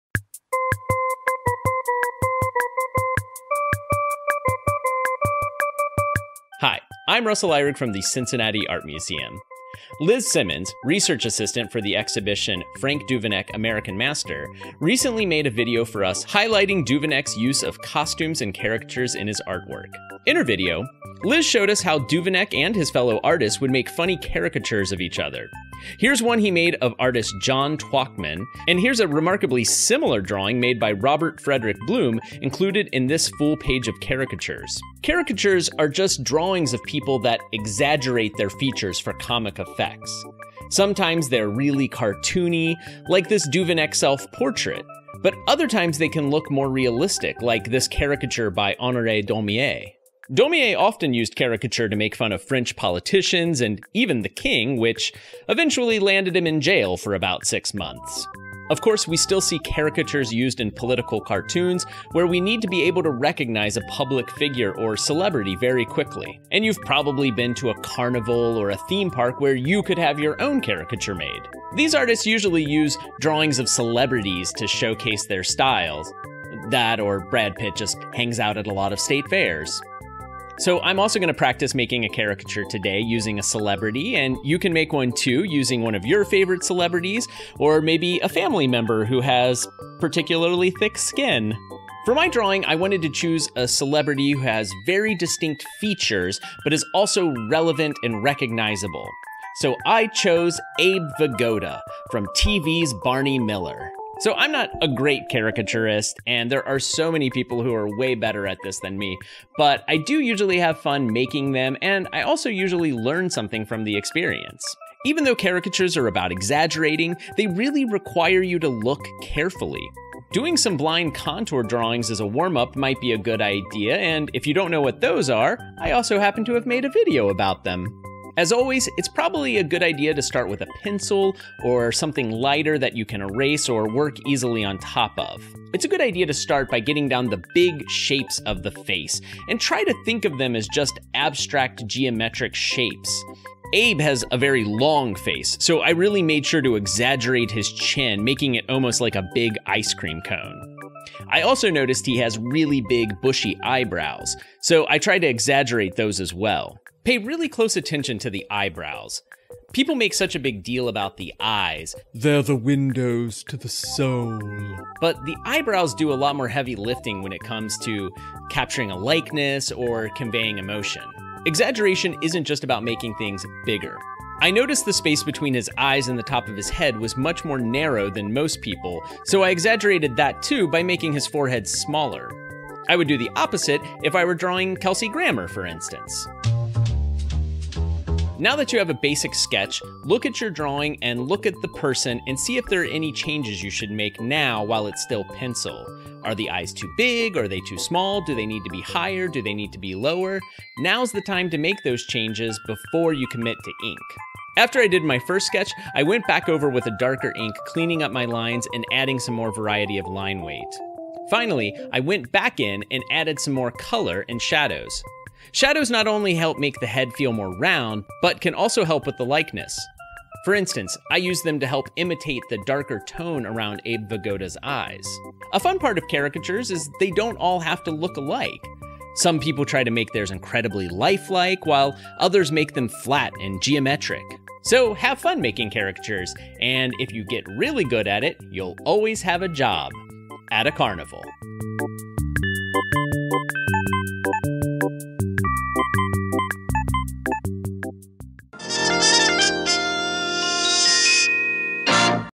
CAM Do | Caricatures | 1/29/21
Learn how artists have used caricatures in the past and create some of your own.